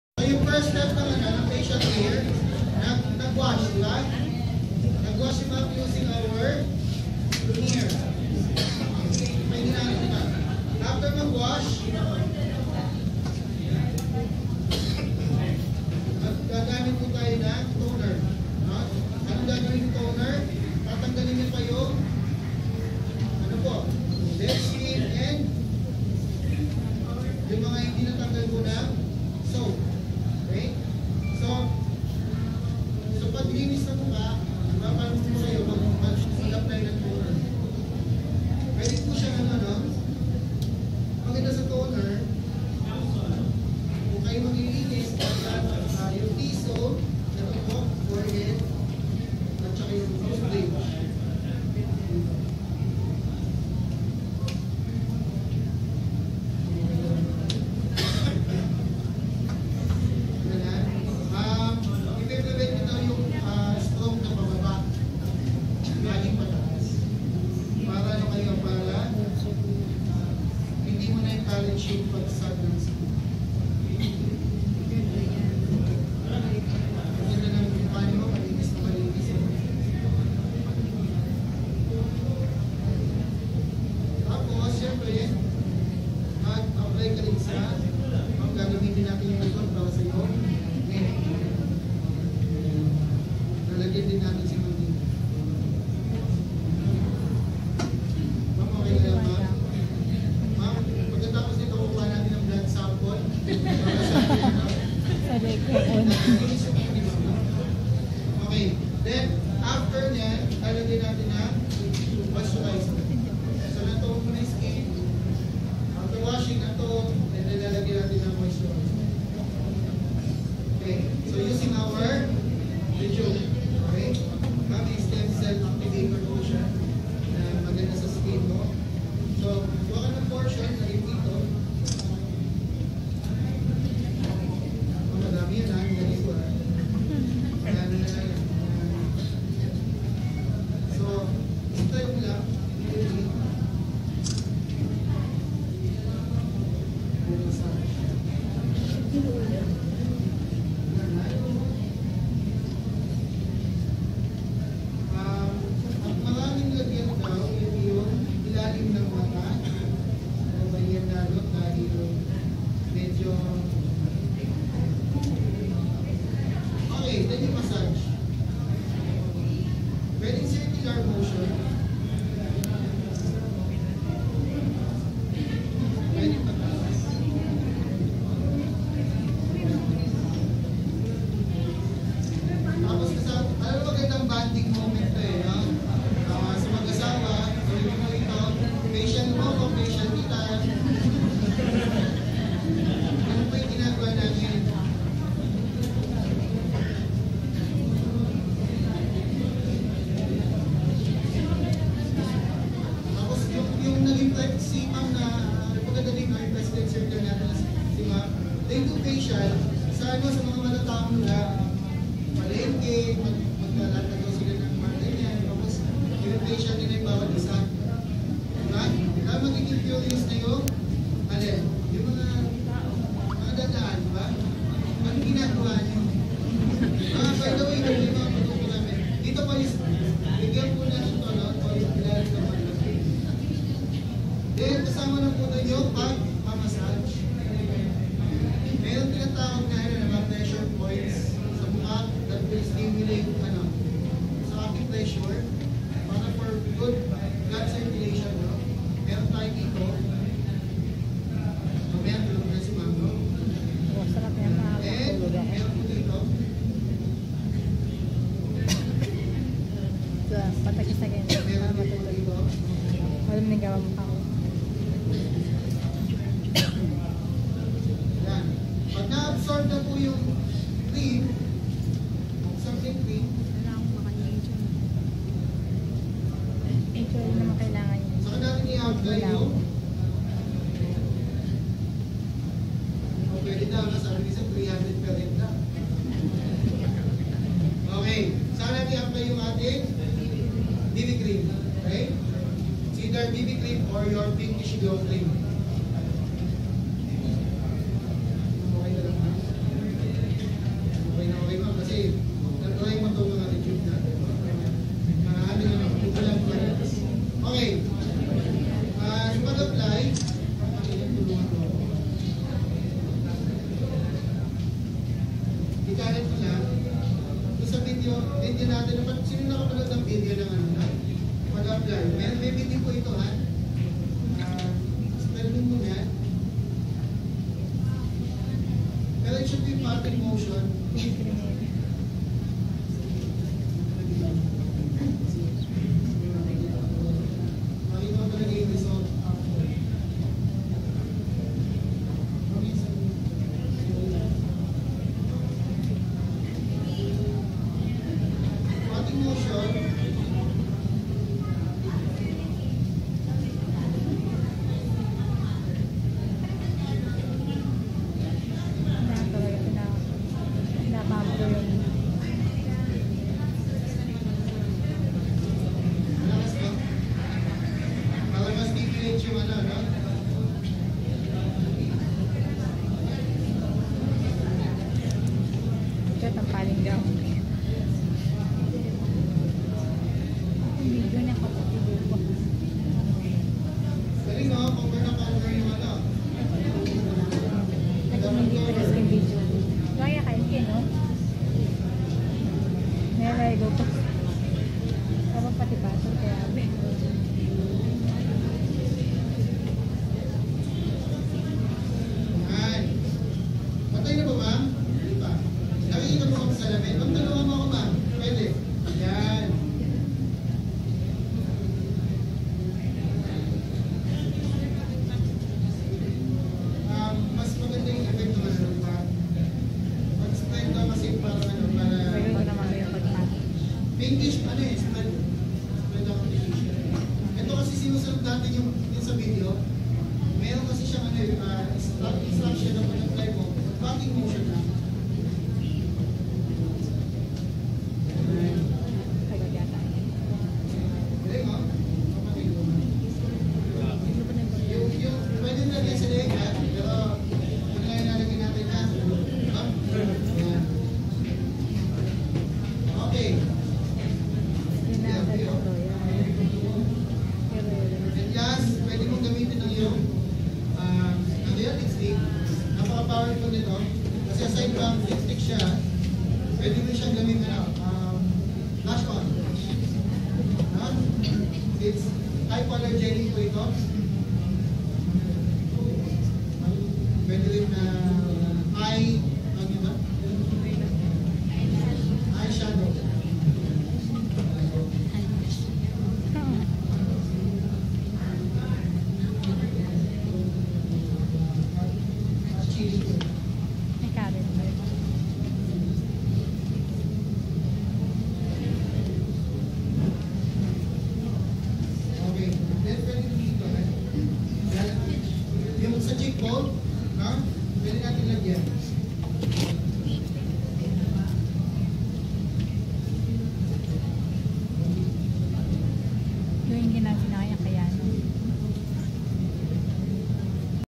Proper Facial Care The ROYALE Way
This is the proper way on how to clean your face & how to put facial ROYALE products..